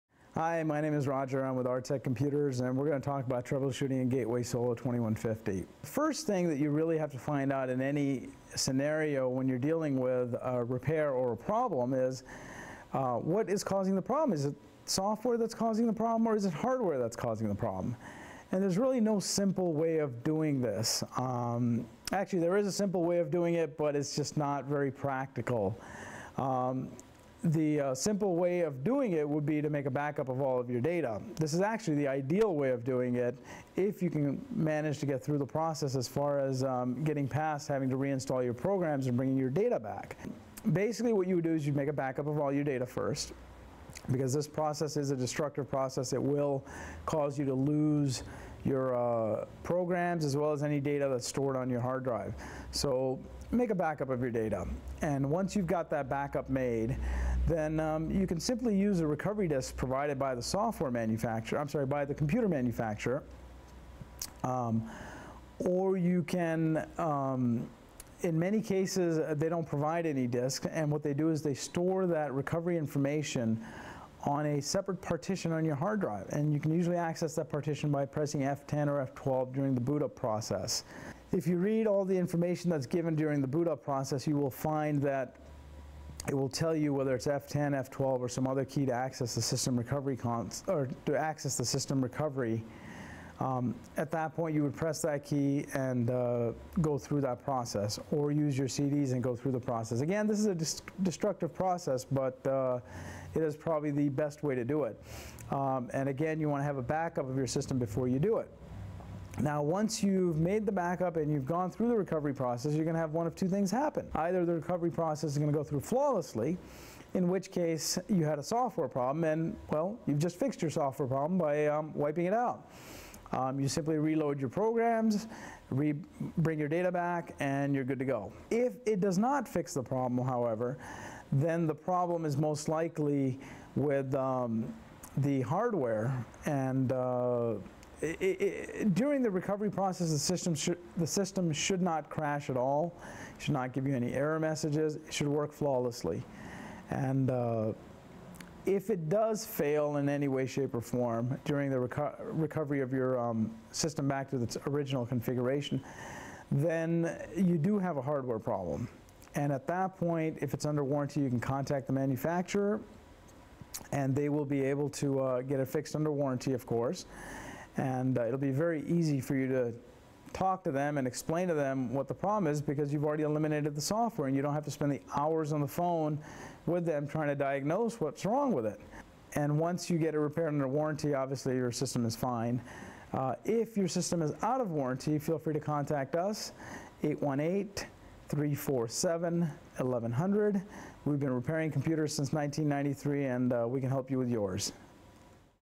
Gateway Computer Troubleshooting : Gateway Solo 2150 Troubleshooting
Gateway Solo 2150 troubleshooting involves backing up all files, reloading the computer back to factory settings and reinstalling all programs to diagnose the problem. Try to fix a Gateway Solo with information from the CEO of a computer and video service company in this free video on computers.